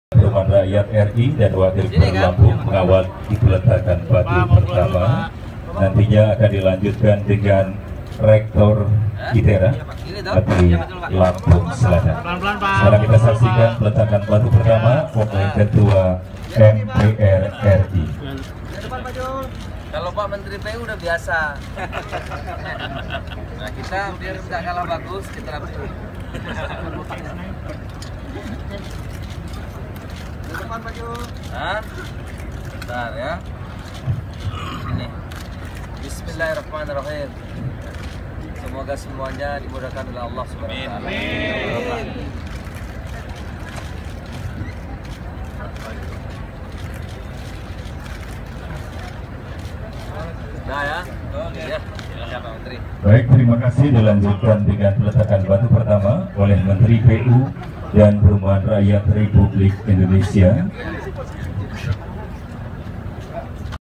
Ketua MPR Zulkifli Hasan Meresmikan Pembangunan Masjid Raya ITERA
Ketua MPR Zulkifli Hasan Meresmikan Pembangunan Mesjid Raya Institut Teknologi Sumatera (ITERA) dengan Peletakkan Batu Pertama, Kamis (6/11)

Untuk info Update terbaru,